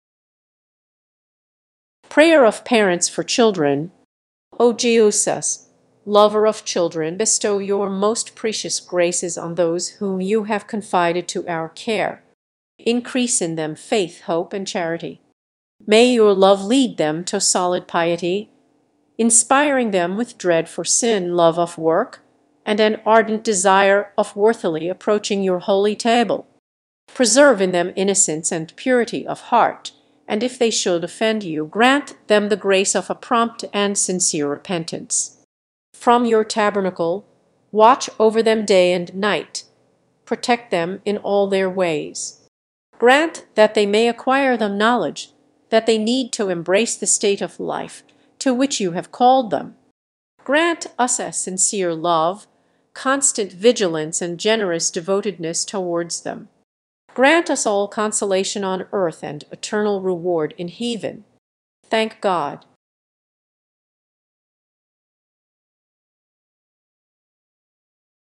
Prayer of Parents for Children ~ Powerful Daily Prayers
Prayer of Parents for Children

O Jesus, Lover of children, bestow Your most precious graces on those whom You have confided to our care. Increase in them faith, hope and charity.

May Your love lead them to solid piety, inspiring them with dread for sin, love of work, and an ardent desire of worthily approaching Your Holy Table.

Preserve in them innocence and purity of heart; and if they should offend You, grant them the grace of a prompt and sincere repentance.

From Your tabernacle, watch over them day and night; protect them in all their ways. Grant that they may acquire the knowledge that they need to embrace the state of life to which You have called them.

Grant us a sincere love, constant vigilance and generous devotedness towards them. Grant us all consolation on earth and eternal reward in heaven. Amen.

prayer, daily prayer, powerful prayers, god, jesus, christ